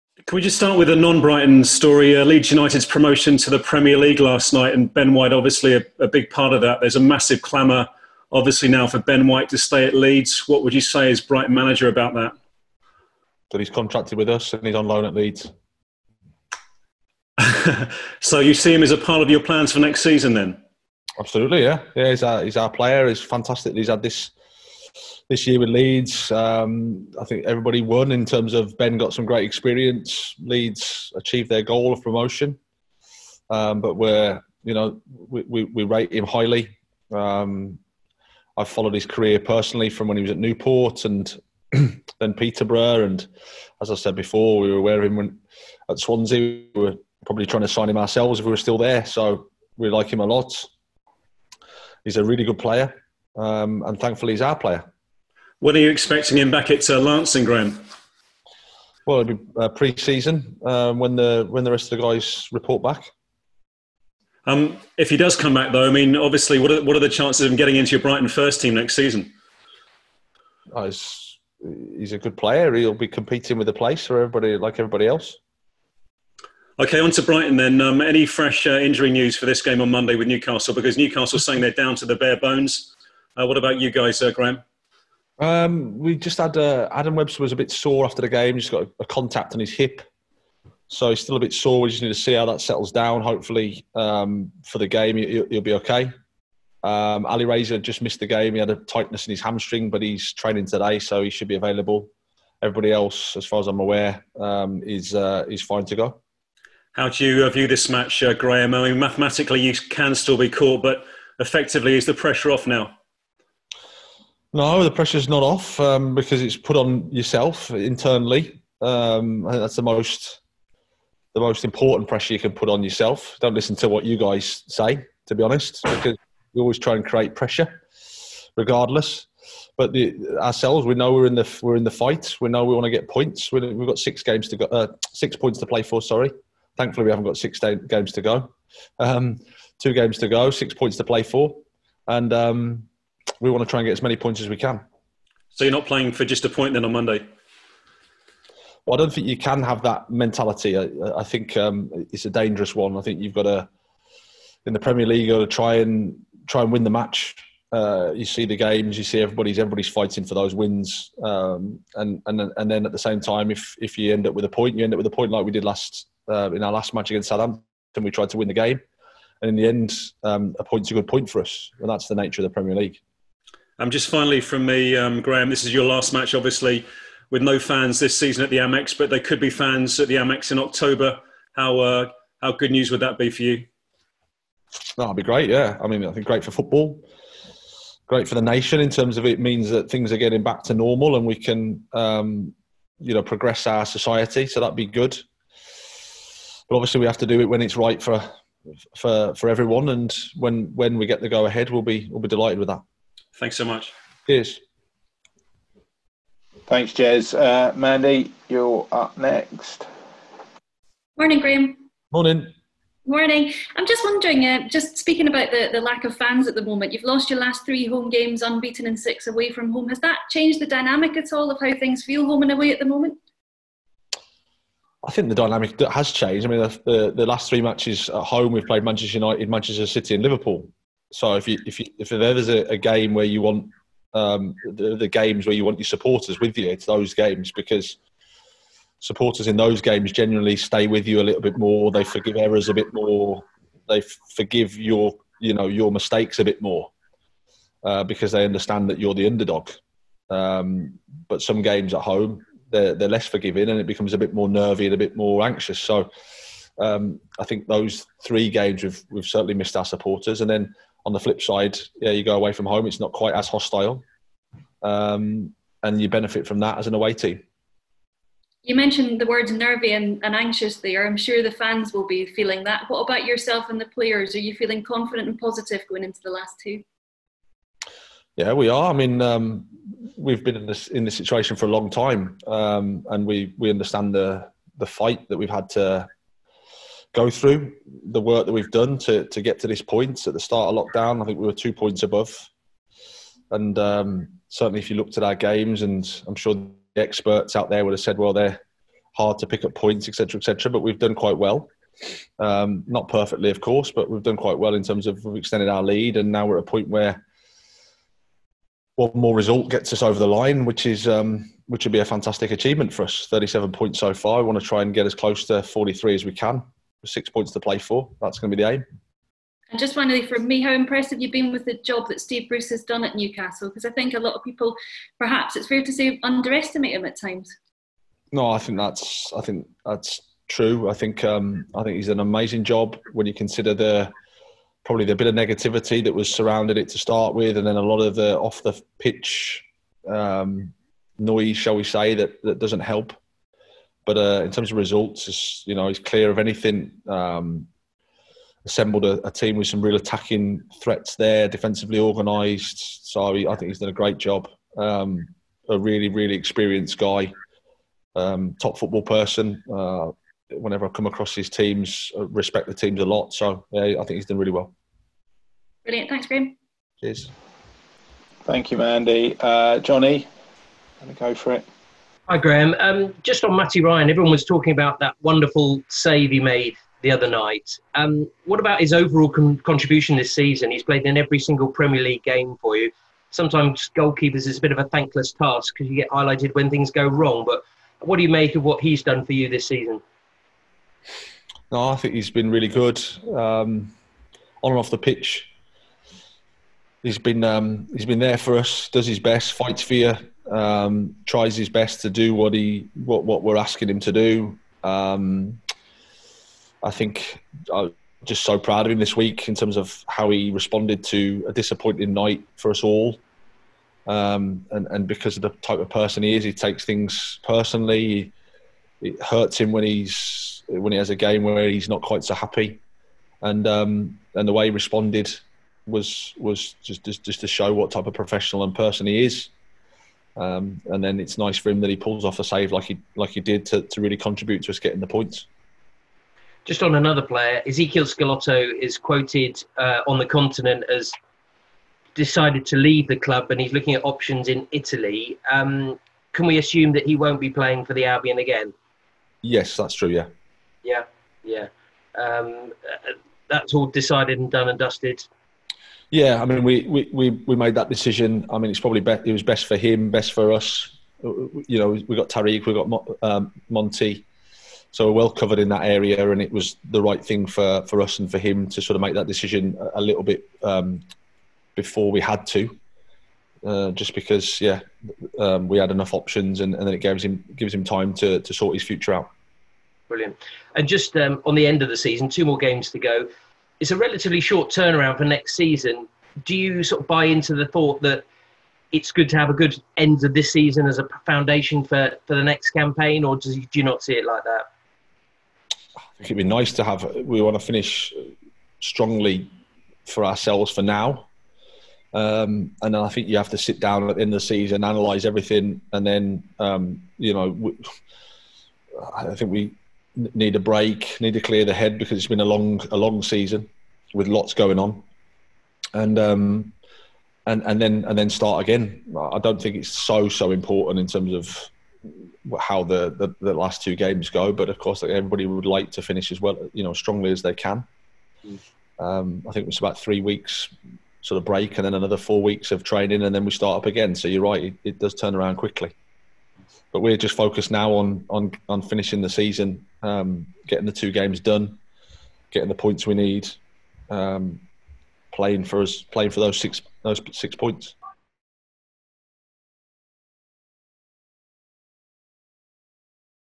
Graham Potter's Newcastle Press Conference
Graham Potter speaks to the media ahead of Albion's last home fixture of the season, where a point can secure Premier League survival.